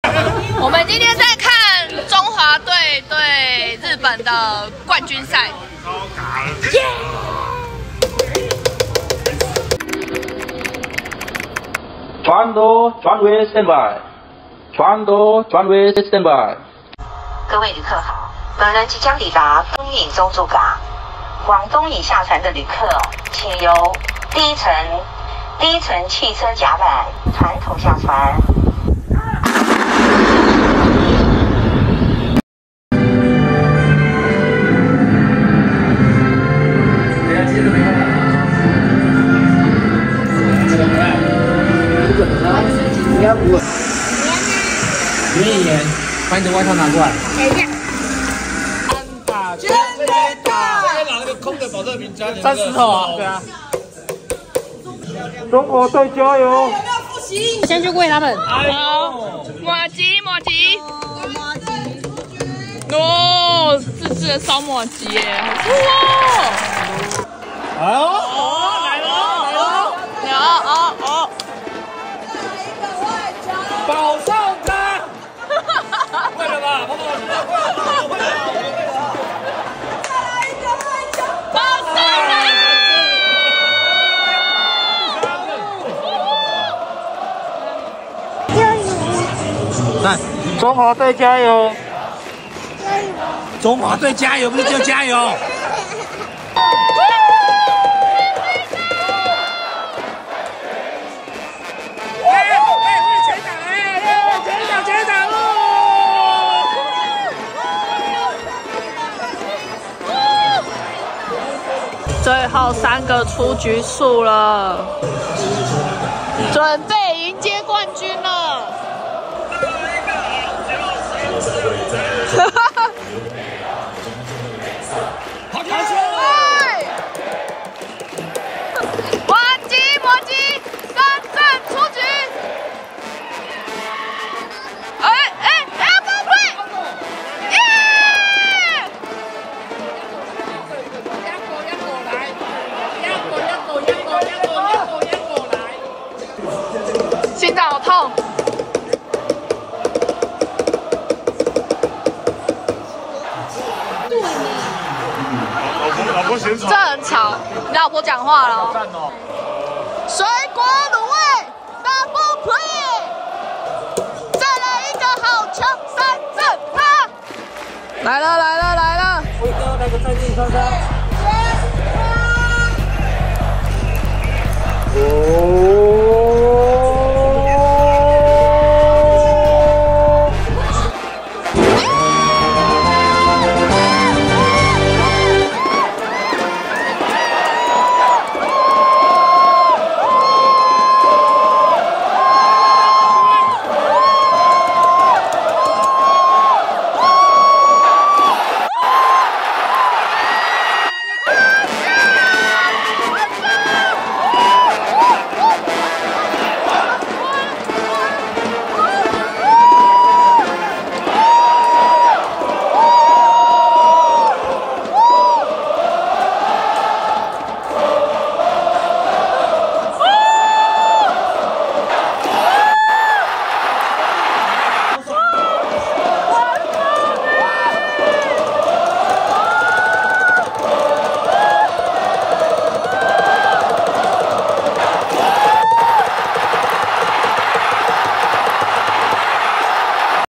東引媳婦日常：鄉代會二樓為中華隊加油｜島民同樂｜說好的全壘打喝三罐｜幾日球迷
這次托東湧社區發展協會的福
能在鄉代會2樓這樣一個大空間
跟島上的朋友們聚在一起為中華隊加油
一群人一起看球賽最好玩的是有人帶口號 帶氣氛
全壘打打出去吶喊到 售票處都聽得到我們的聲音
甚至還有島上隱藏版高手特製燒麻糬
呼應應援口號「麻吉麻吉 三振出局」！！！
影片雖然短 但就是想記錄大家聚在一起的時光與氣氛